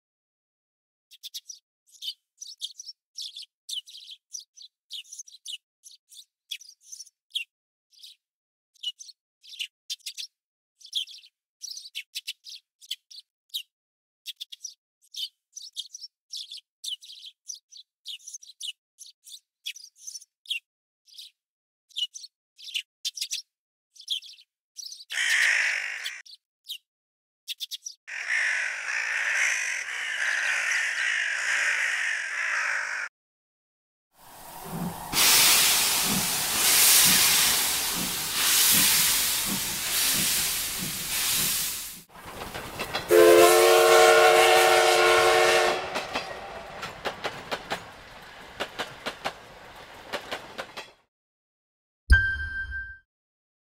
4K OLED OBJECT SOUND TRACKING DEMO (SONY)
A video demonstrating object tracking sound in 4k for Oled screens.

All rights owned by SONY